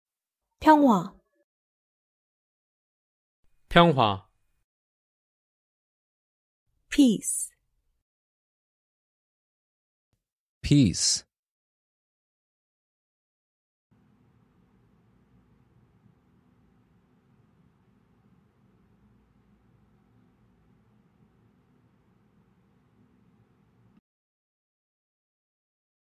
How to say "Peace" in Korean
Male Voice:
평화

Female Voice:
평화

Male Voice Translation:
Peace

Female Voice Translation:
Peace